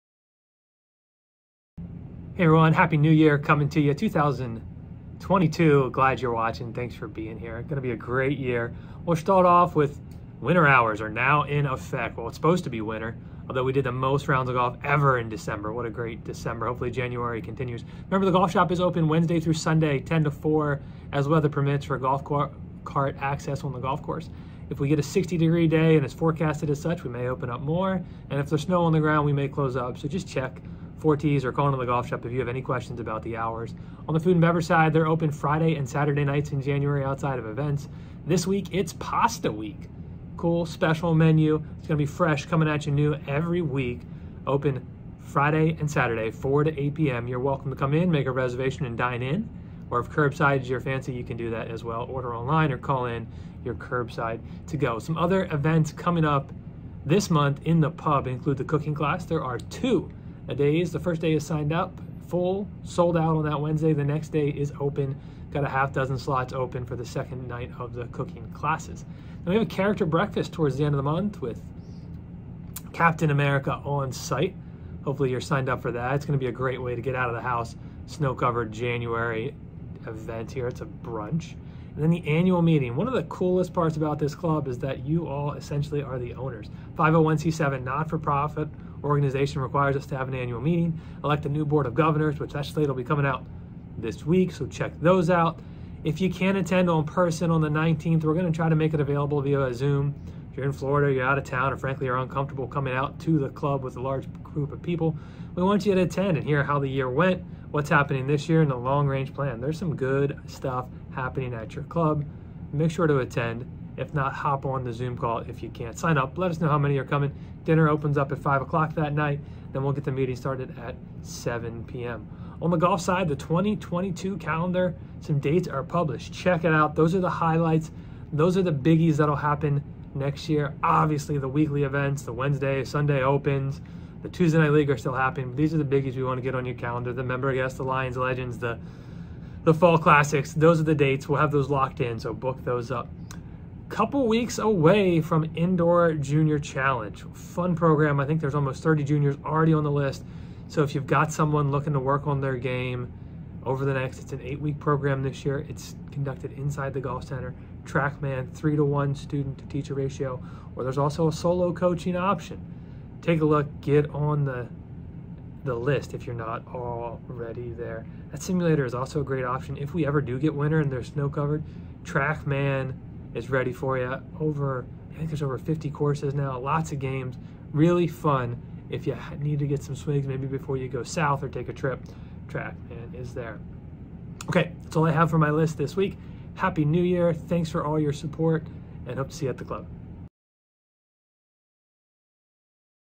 Weekly Video Update Jan 6
Weekly Update for Jan 6. Click to keep up to date with Chambersburg Country Club.

Highlights this week include January Club Hours, Upcoming Events and other news around the Club.